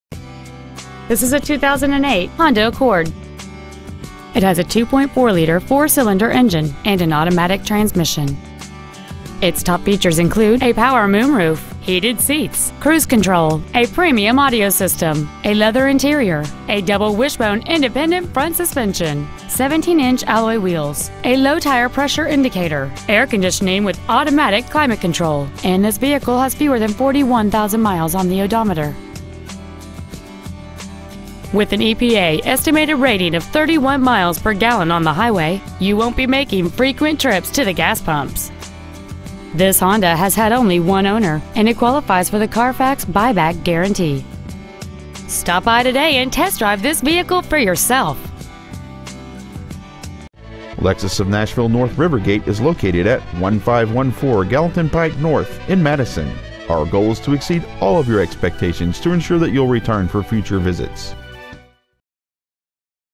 Pre-Owned 2008 Honda Accord Sdn Madison TN
Year: 2008
 Make: Honda
 Model: Accord Sdn
 Engine: 2.4L DOHC MPFI 16-valve i-VTEC I4 engine
 Trans.: Automatic
 Exterior: Silver
 Miles: 40,200
 Interior: Gray
 Stock: N103061A

 Used 2008 Honda Accord Sdn Madison TN